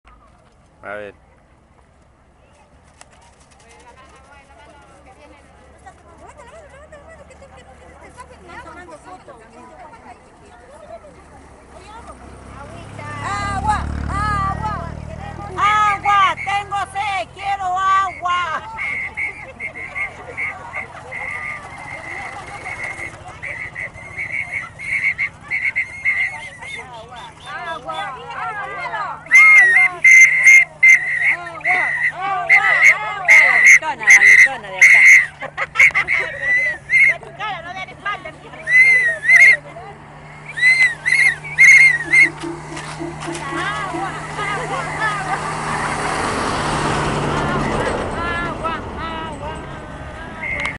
Ventanilla Cadena Humana por Agua
pobladores de Centro Poblado Mi Peru exigen ejecución de proyecto de agua potable por gobierno nacional
cortesia crecercallao